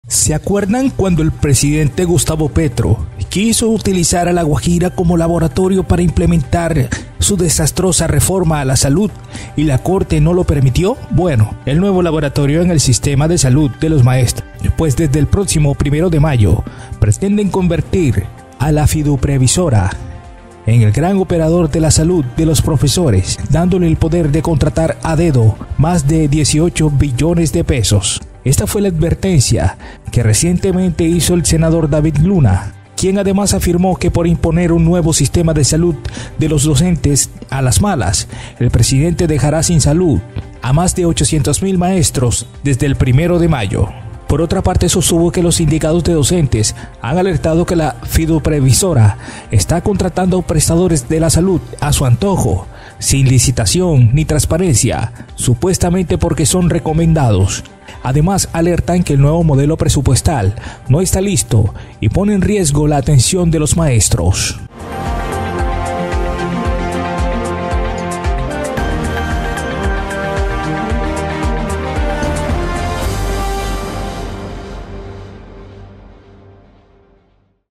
“Gustavo Petro dejará sin salud a más de 800.000 maestros desde el 1 mayo”: David Luna
LaLenguaTv 🎥 | Esta fue la advertencia que recientemente hizo el senador David Luna, quien además afirmó que por imponer un nuevo sistema de salud de los docentes a las malas, el Presidente dejará sin salud a más de 800.000 maestros desde el 1 de mayo.